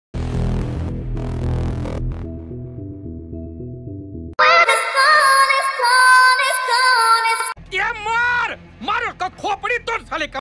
Cockroach ko kaise maare 😜🤯Best technique
It's a comedy short and it's based on cockroach kill dance techniques funny video with famous dialogue of khopdi Tod saale ka meme